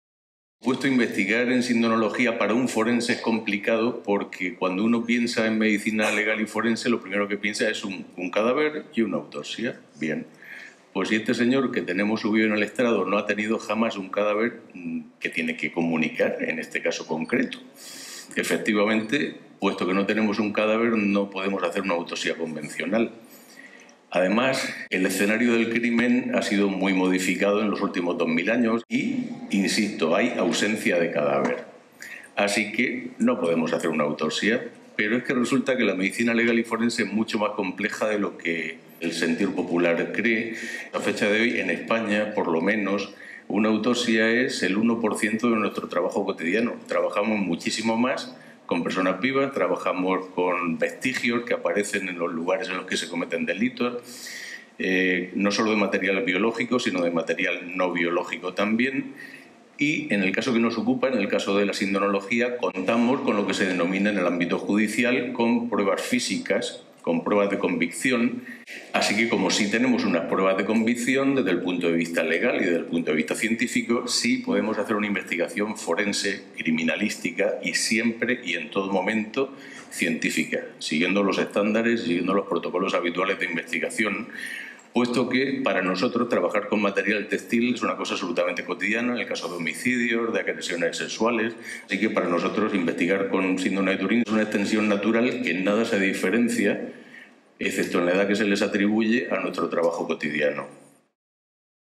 La Síndone de Turín, una autopsia no convencional
Ponencia del Dr. Sánchez Hermosilla, del Instituto de Medicinal Legal y Ciencias Forenses de Murcia.